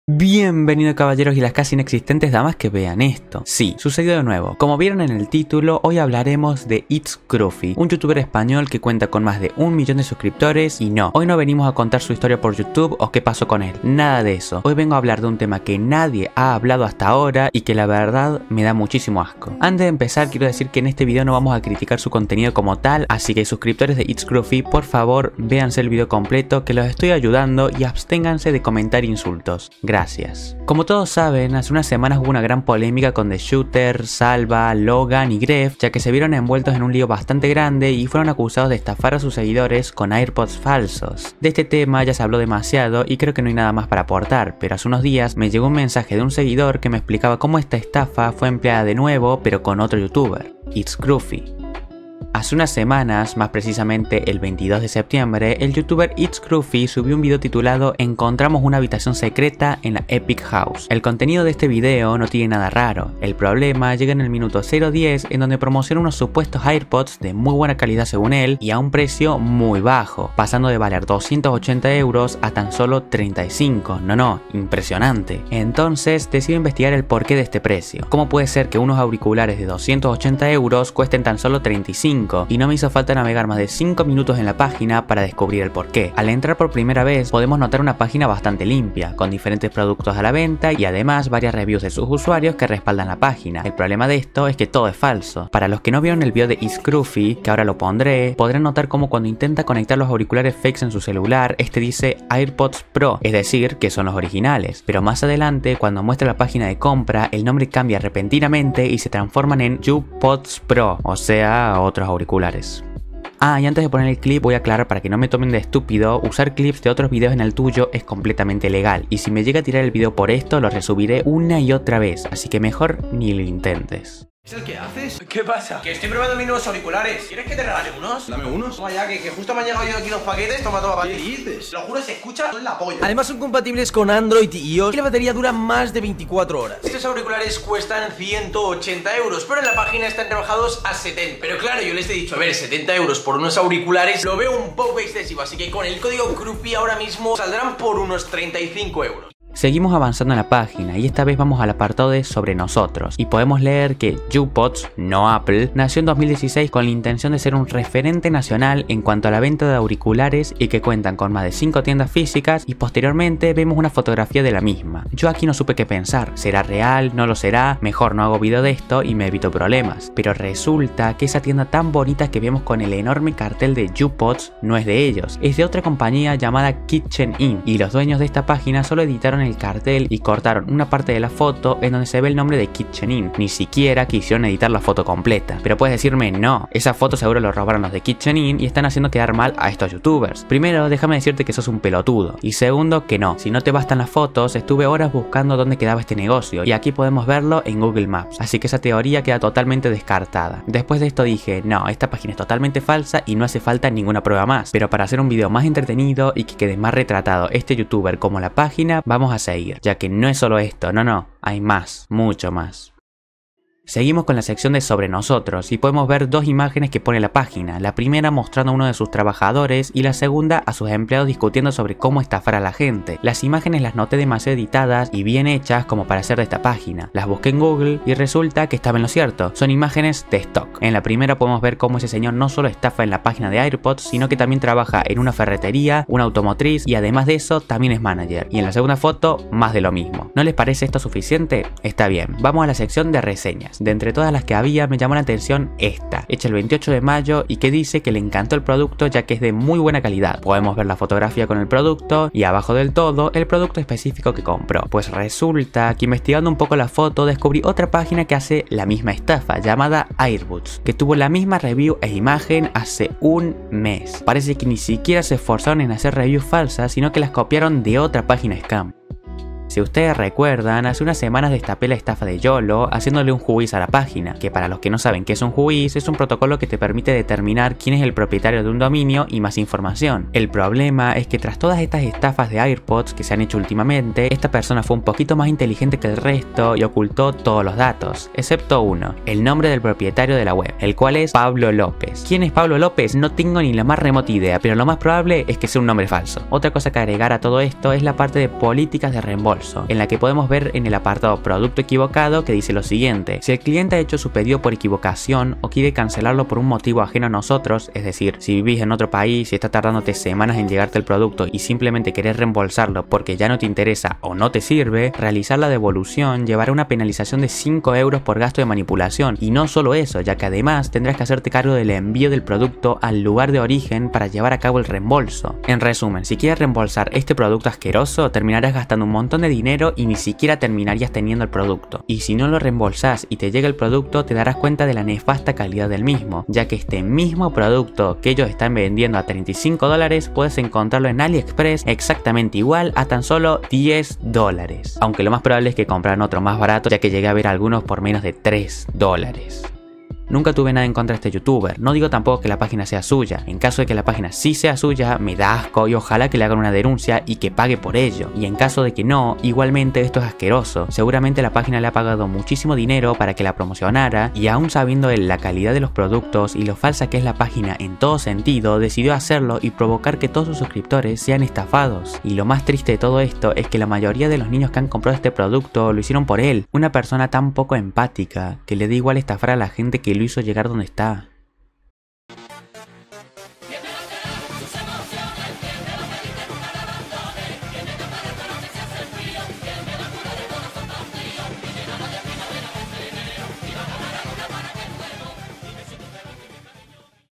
itsKrufy y su ESTAFA de AirPods
Hoy vengo a hablarles sobre la gran estafa por parte del youtuber itsKrufy hacia sus seguidores, intentándoles vender unos Airpods de Apple que terminaron siendo de otra marca pirata (YouPods) y de nefasta calidad.